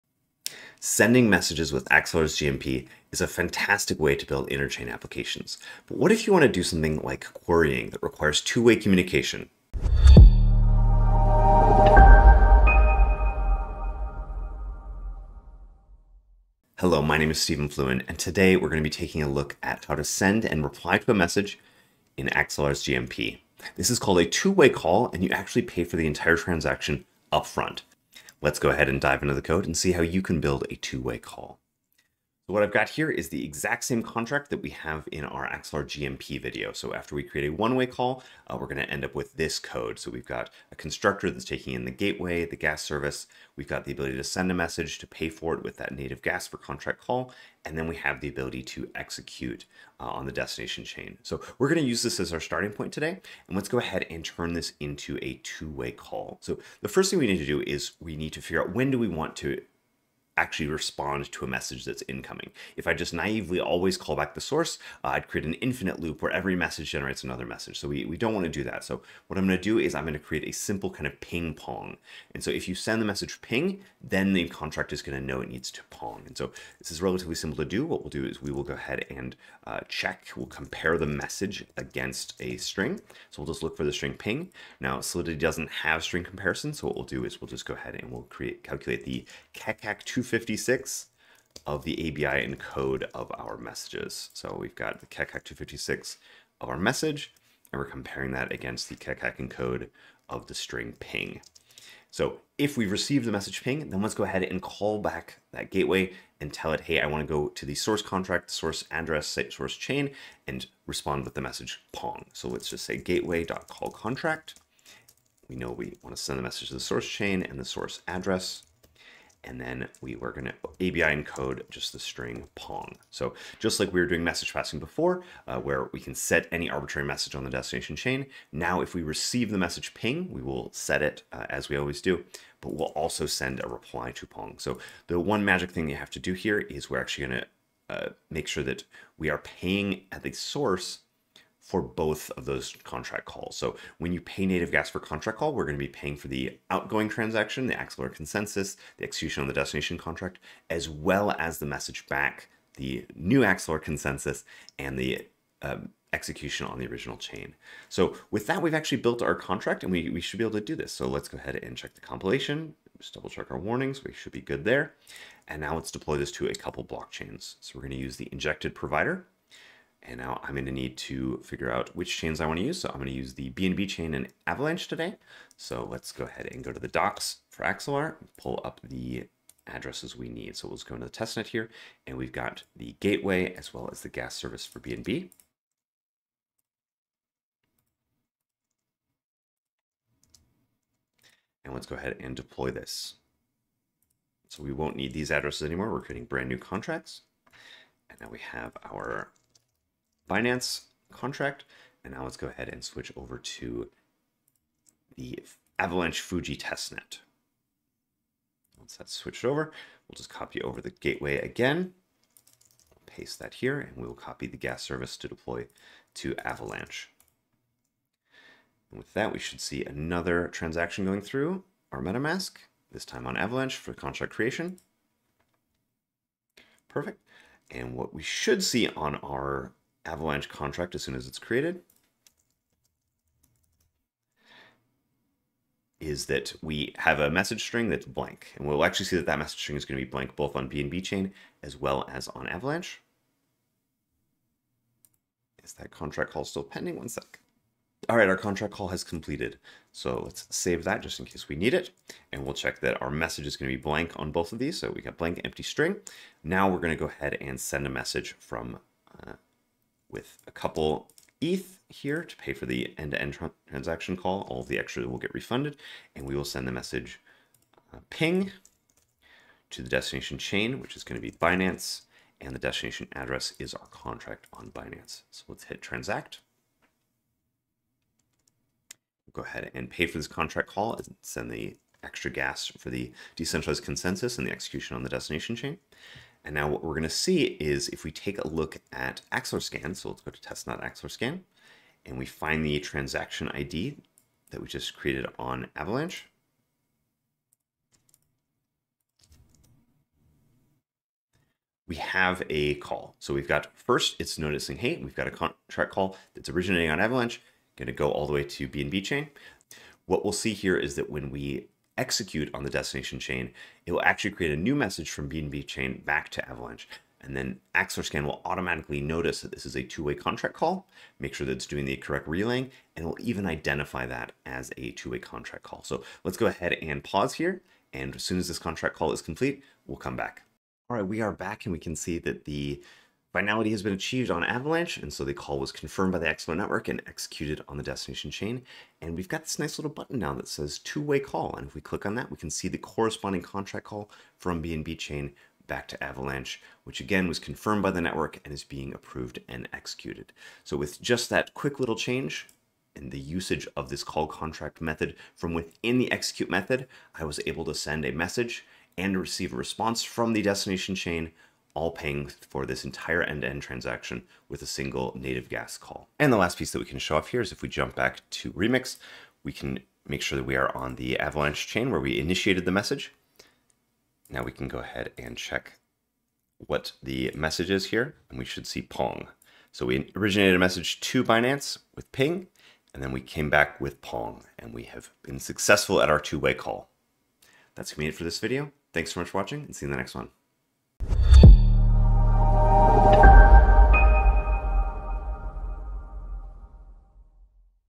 2-Way Calls Using General Message Passing - Axelar Developer Tutorials
Whether you are querying data on another chain, or storing the results of a cross-chain message, you need to create a two-way call with General Message Passing (GMP). This tutorial walks through modifying any existing app using Axelar GMP to send a message back to the source chain without requiring an additional destination chain gas payment.